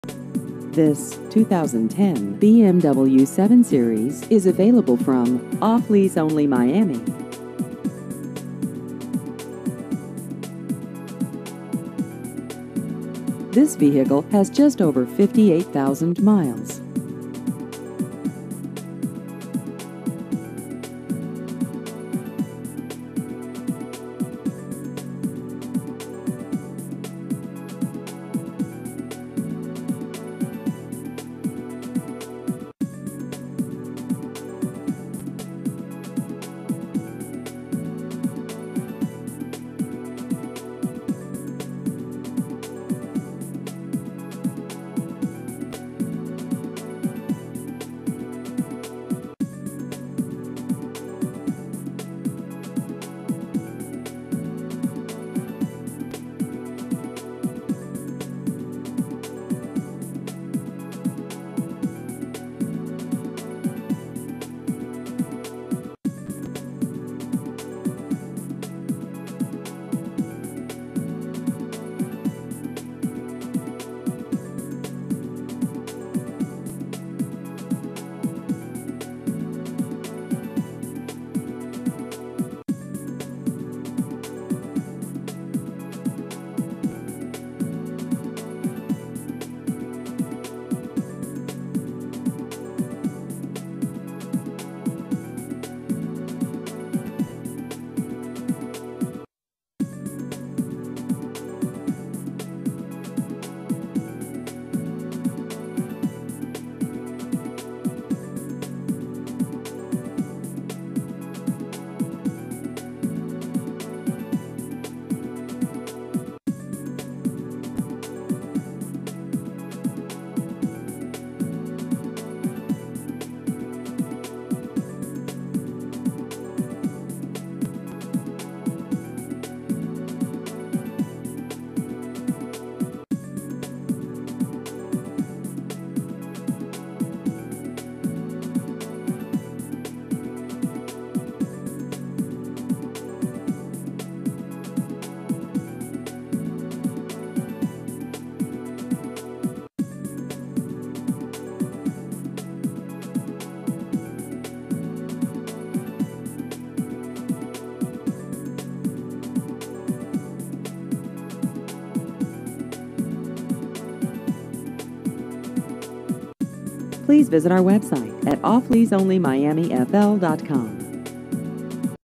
2010 BMW 7 Series Used Cars Miami FL
This 2010 BMW 7 Series is available from Off Lease Only Miami.